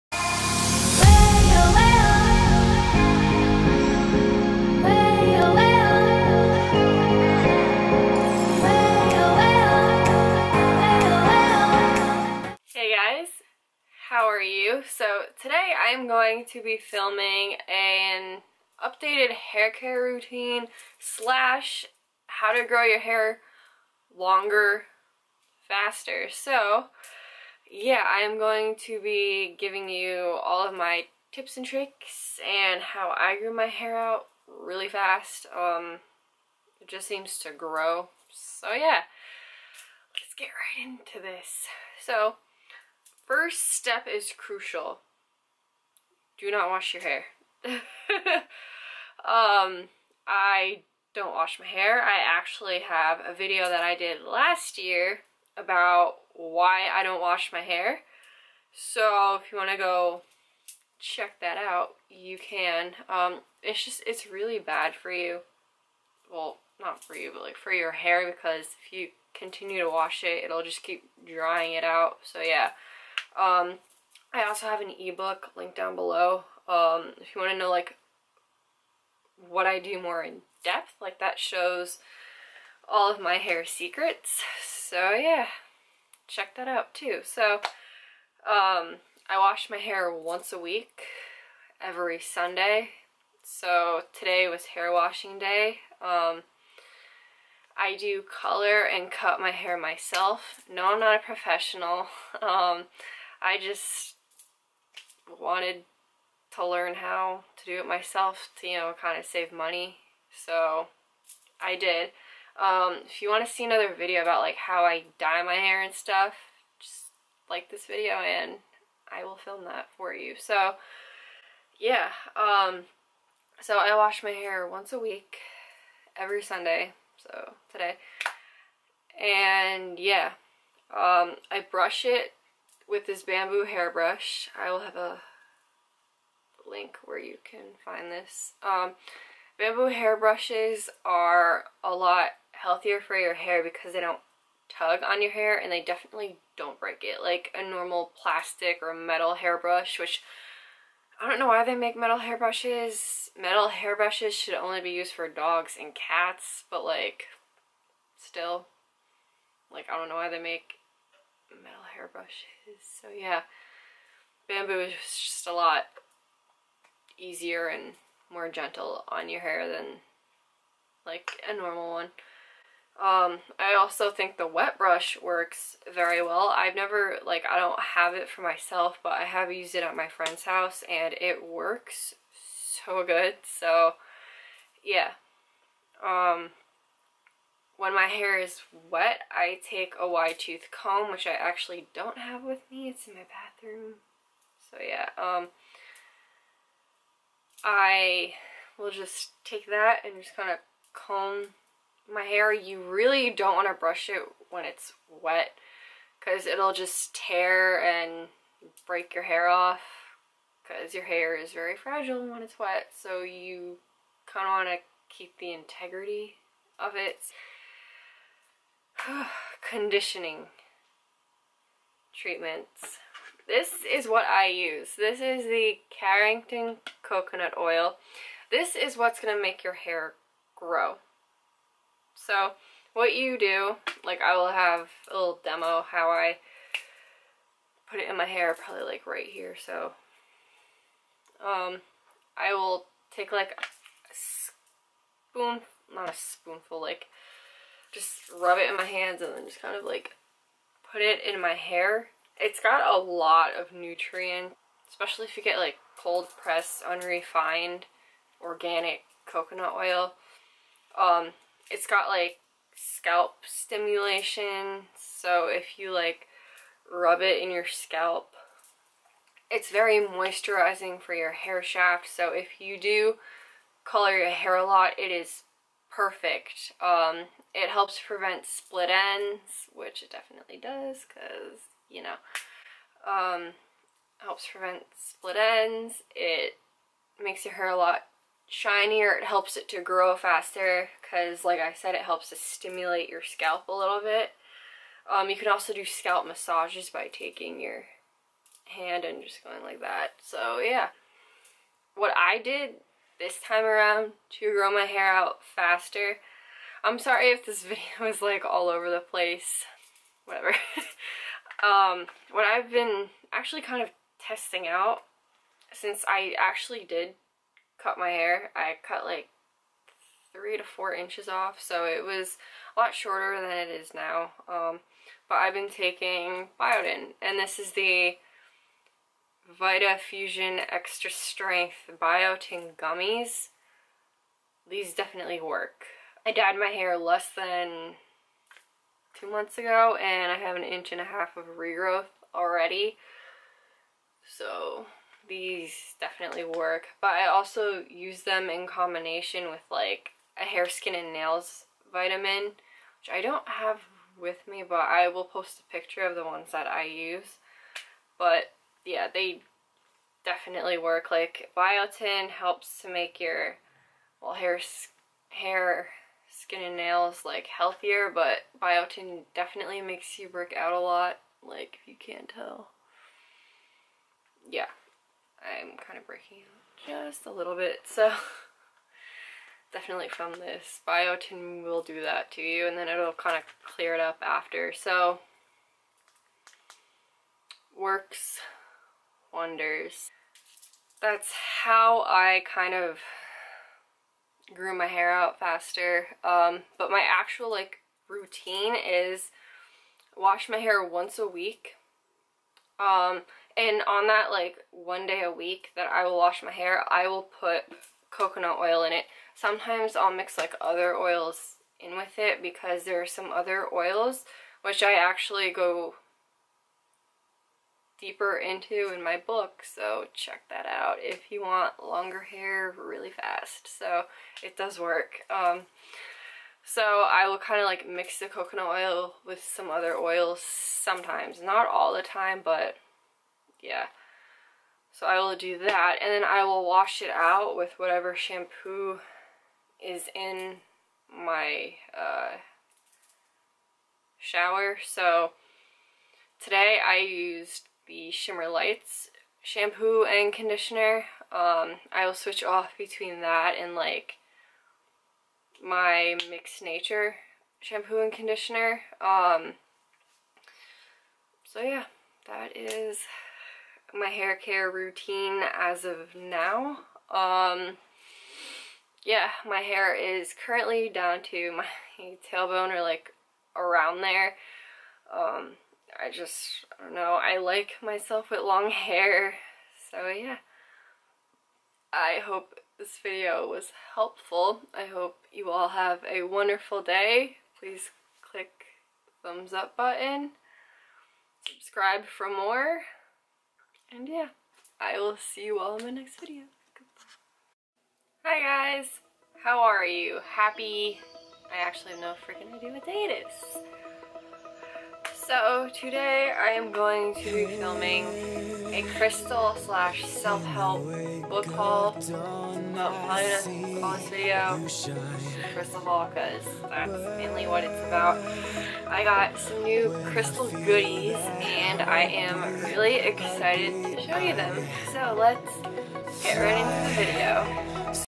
HAIR CARE ROUTINE 2017
OPEN ME!
My Ebook with all my Hair secrets for long hair

Hey guys, in this video I show you what my current hair care routine is and my secret to growing it out even FASTER this time. i really hope this hair care routine was helpful, thank you so much for watching wishing you all a happy and healthy Sunday.